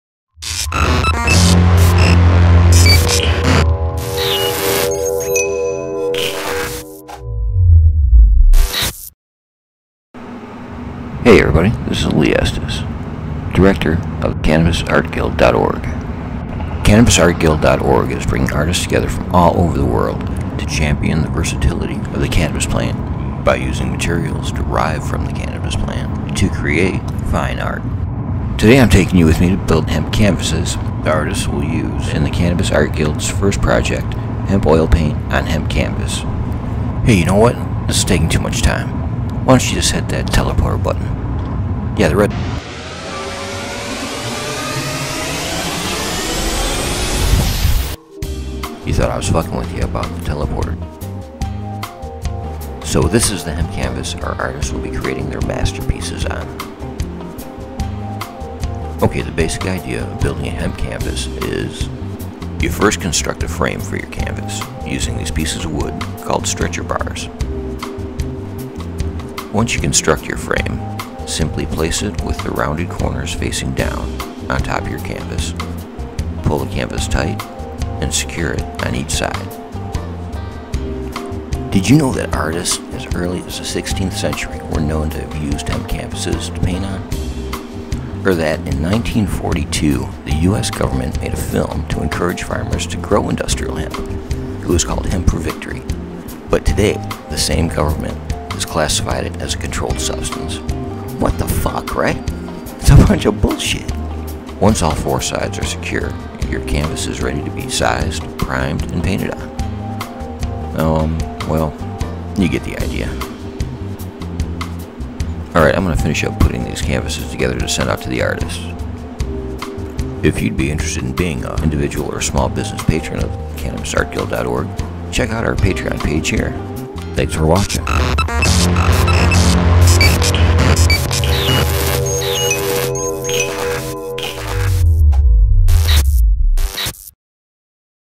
CannabisArtGuild org Introduction : Uncensored
The epic "Art House" introduction to CannabisArtGuild.org featuring  CannabisArtGuild.org Guild Director, Lee Estes.

Artists from around the World are coming together to champion the versatility of Cannabis!

By bringing artists together from a list of countries, hopes to unite a number of creative voices internationally to deliver one strong statement globally on the flexibility and versatility of the Cannabis plant.

Using materials derived from the cannabis plant, these artists will stretch the limits of their creativity along with Society's preconceived notions in, creating works of fine art.